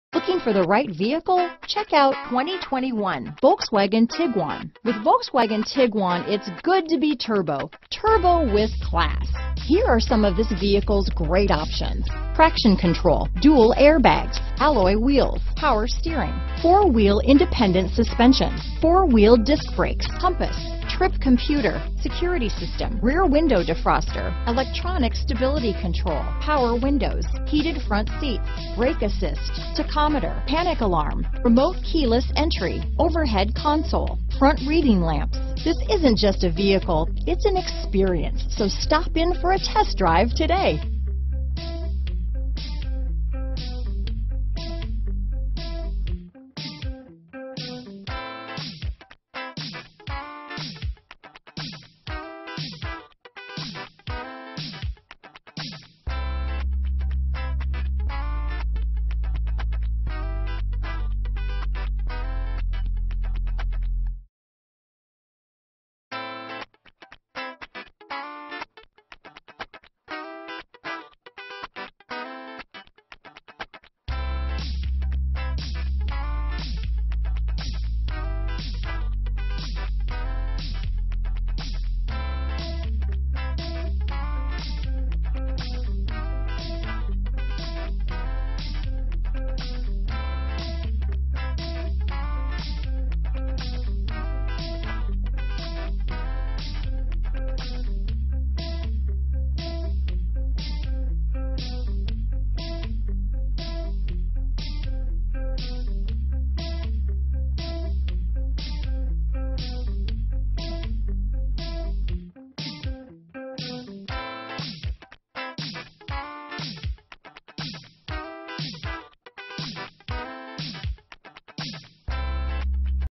2021 Volkswagen Tiguan Las Vegas, Henderson, North Las Vegas, Boulder City, Mesquite, NV M048238
Deep Black Pearl New 2021 Volkswagen Tiguan available in Las Vegas, Nevada at Findlay North Volkswagen. Servicing the Henderson, North Las Vegas, Boulder City, Mesquite, NV area.
2021 Volkswagen Tiguan 2.0T SE - Stock#: M048238 - VIN#: 3VV3B7AX7MM048238

Prices include all applicable incentives and are not compatible with subvented rates through VCI.